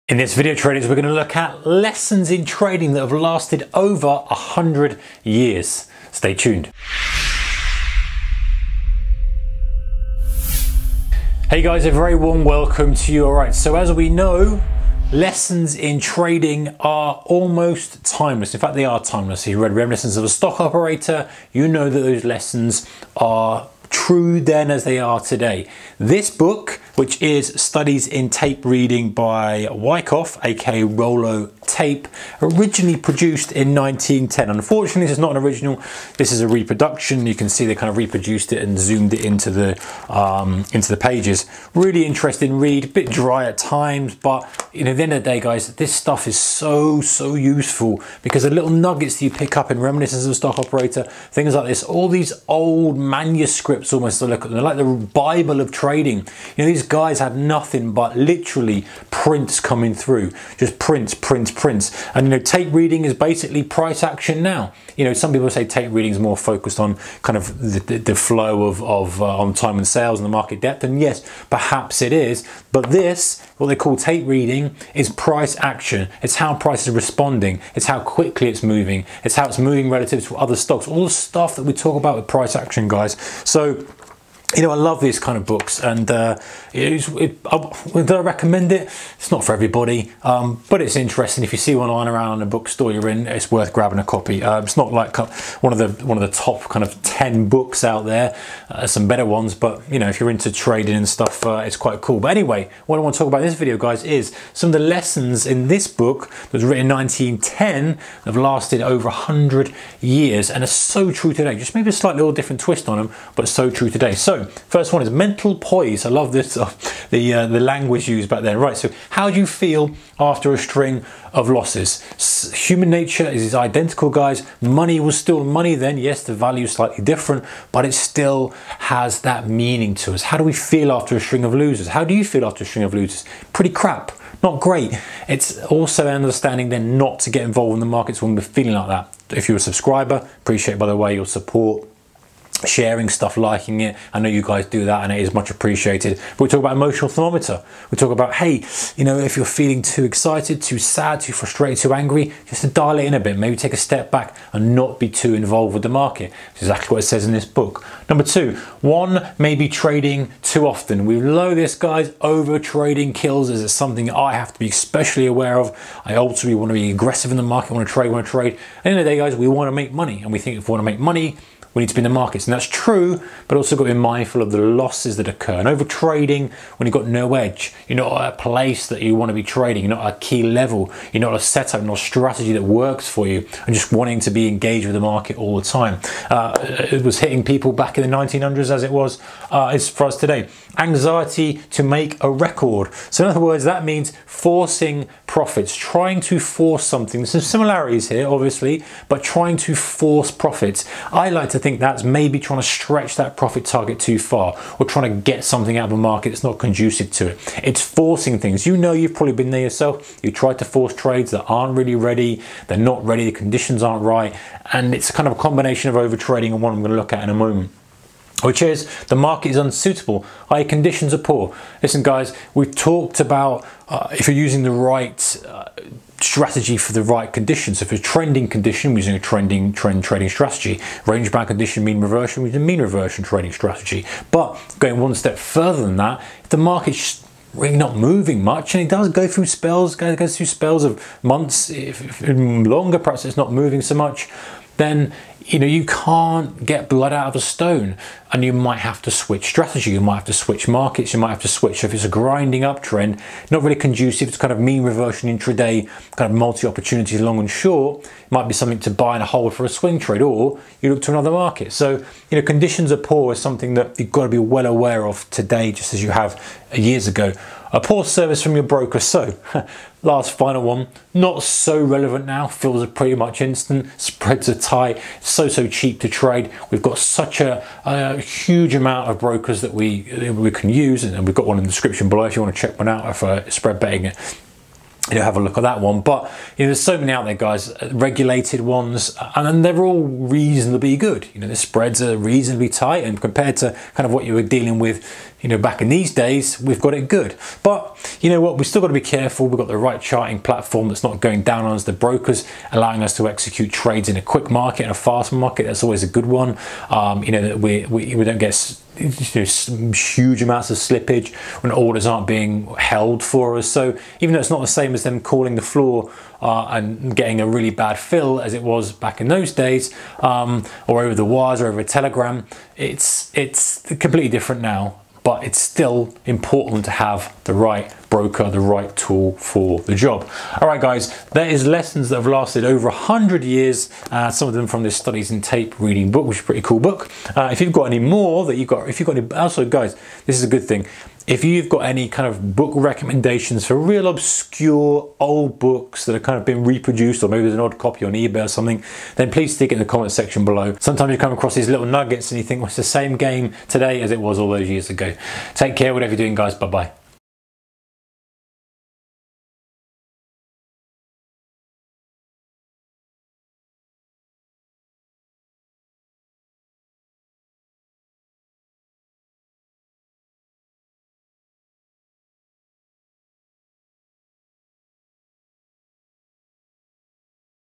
Lessons in Trading that Have Lasted over 100 Years ☝️
If you have read Reminiscences of a Stock Operator or Studies in Tape Reading by Wyckoff you will realise that most of the challenges traders of old times experiences are still true today.
1) Mental poise - How do you feel after a string of losses.
2) One may be trading too often - overtrading/no edge.
3) Anxiety to make a record - forcing profits.
4) The market is unsuitable - conditions are poor.
5) Poor service from your broker - a decent platform.
emotional barometer video OK.